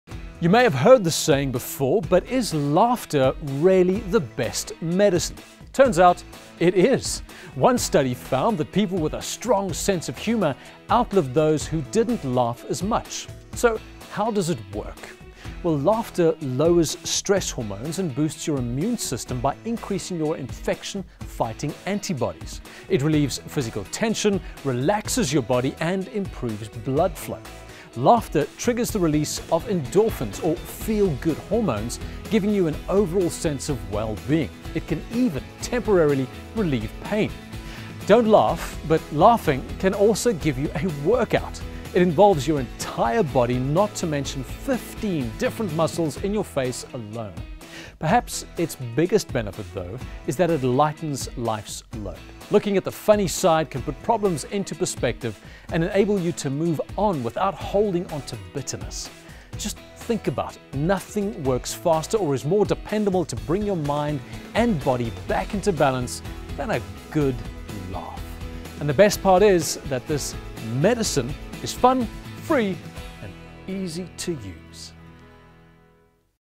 Science explains why laughter is the best medicine
Science have shown how laughter can affect your health. Dr. Michael Mol tells us more.